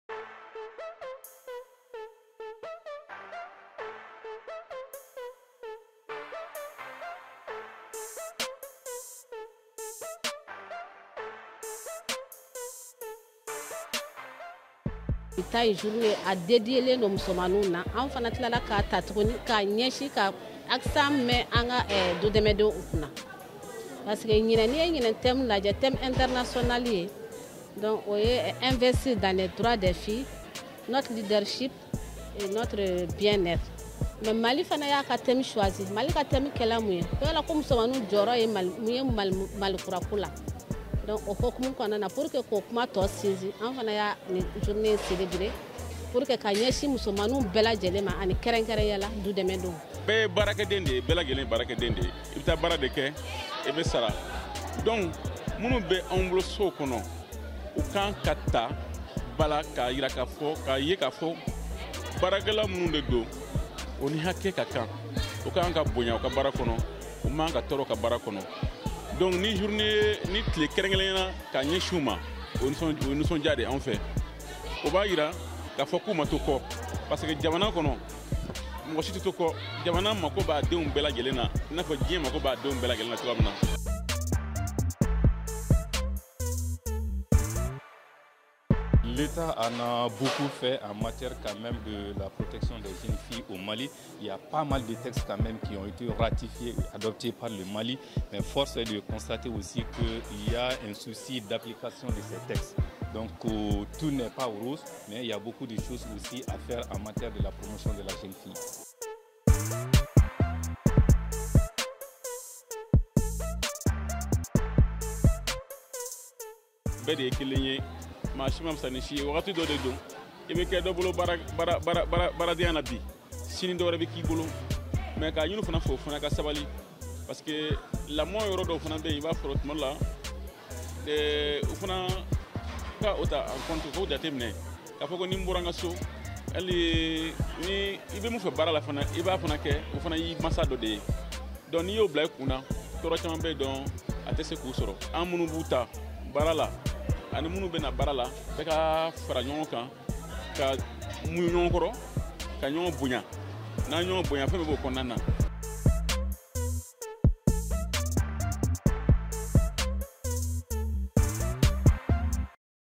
Les travailleuses domestiques à l'honneur
Le consortium COMADE, GRADEM et SOLI AM, en collaboration avec la Direction Régionale de la Promotion de la Femme, de l'Enfant et de la Famille, a organisé une initiative le jeudi 19 octobre 2023 à la Maison de la Femme de Bamako, à l'occasion de la Journée Internationale de la Jeune Fille. Le thème de cette journée était "Le rôle des Filles Travailleuses Domestiques dans la Refondation de l'État Malien".